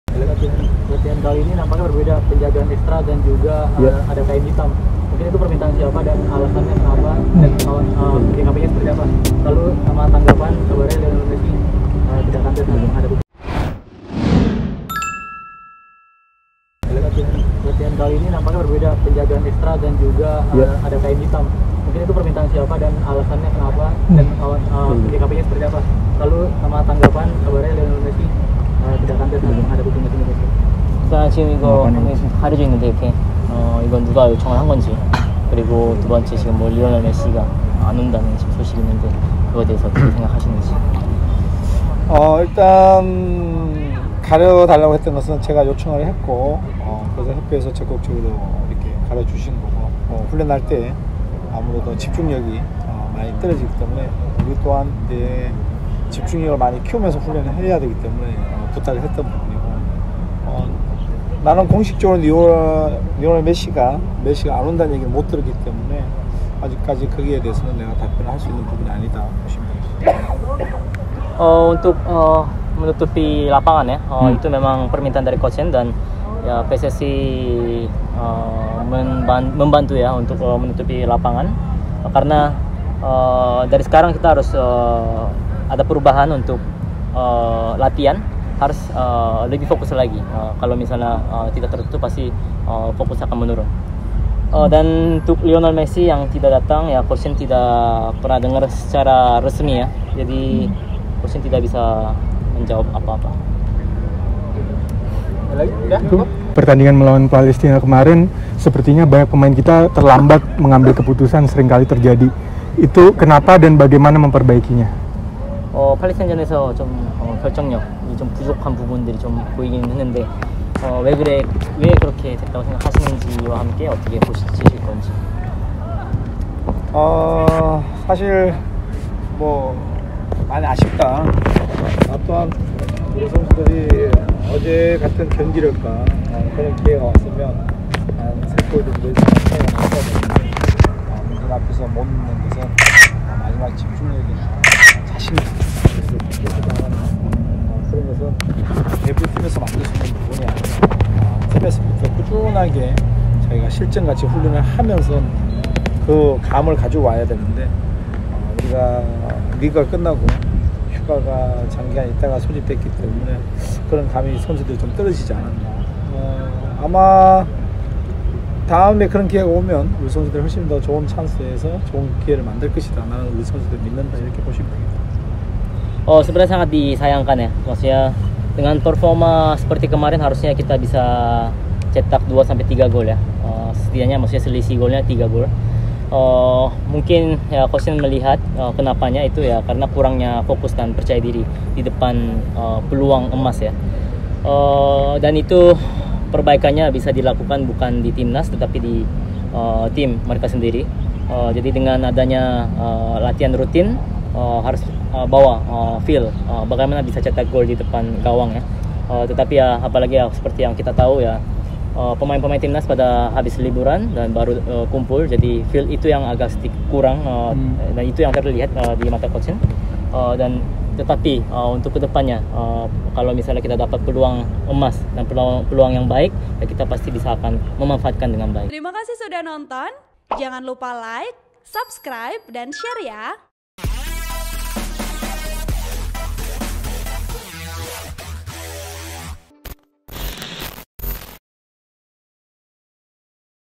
FIFA MATCHDAY | Tanggapan STY Terkait Messi Batal Main & Evaluasi Timnas Jelang Lawan Argentina
Tanggapan Shin Tae-yong Terkait Lionel Messi Batal Main & Evaluasi Timnas Indonesia Jelang Melawan Timnas Argentina

(NSA)